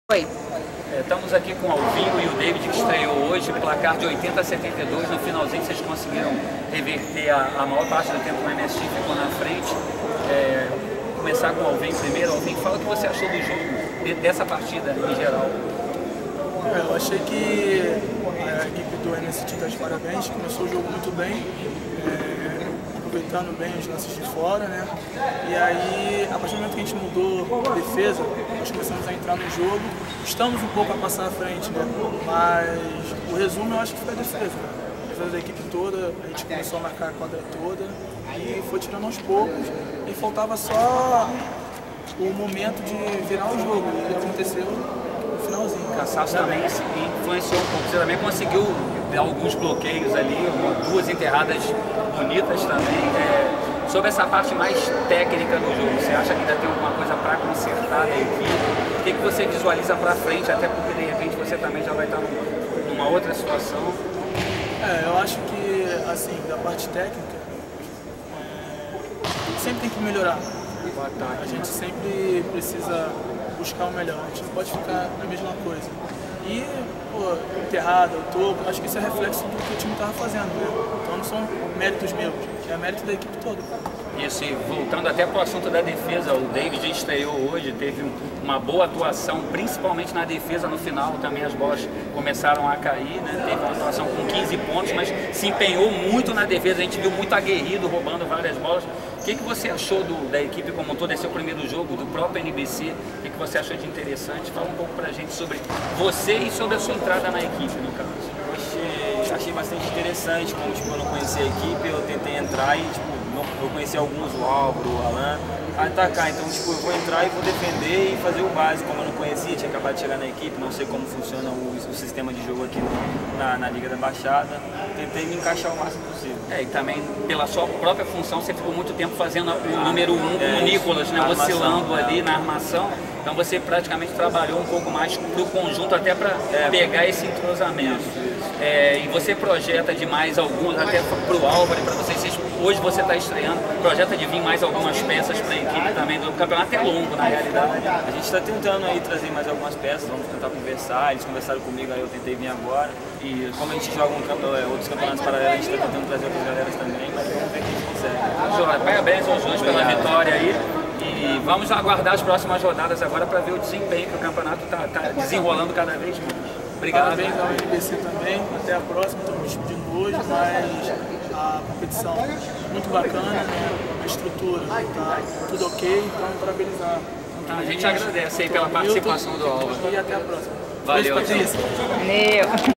NBC2 - NOVO BASQUETE CLUBE - 11ª Semana - Bad Angels - Alvinho e David
Entrevista com os destaques da equipe do Bad Angels Alvinho e David ... Ambos respectivamente marcaram 26 e 15 pontos ... Sendo 32,5 e 18,75% dos pontos total da equipe !!!
Eles relatam que realmente houve uma dificuldade muito grande na vitória pela ótima partida realizada pelo MS TEAM
Ambos tiveram uma grande atuação na defesa com vários tocos de Alvinho e 2 bonitas enterrada e a estréia de David com muita aplicação na defesa para que depois possa pegar entrosamento com todos os outros atletas de sua nova equipe !!!